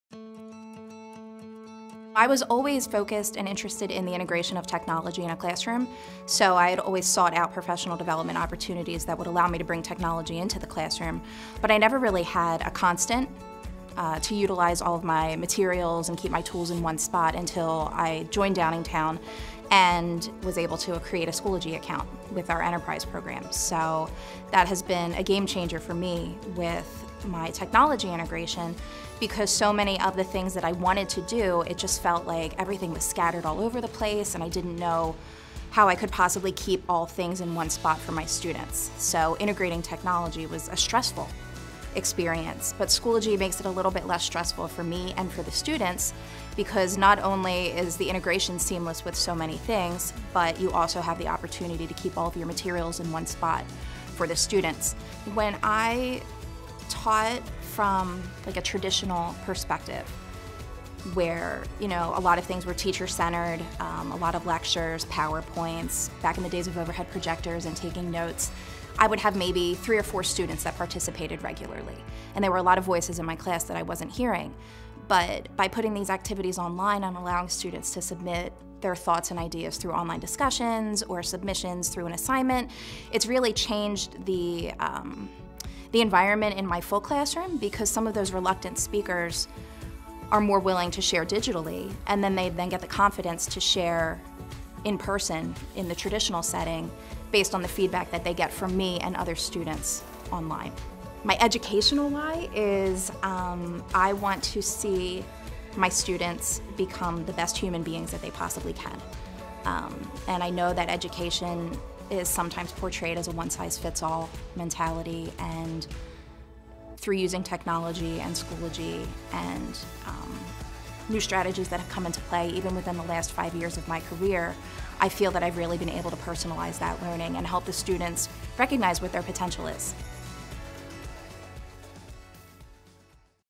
Denise Shovelin | Technology Integration in the Classroom
Denise Shovelin is the English teacher and Tech Innovator at Downingtown Area School District. She talks about how easy it is to integrate technology in the classroom and how it's helped empower her students.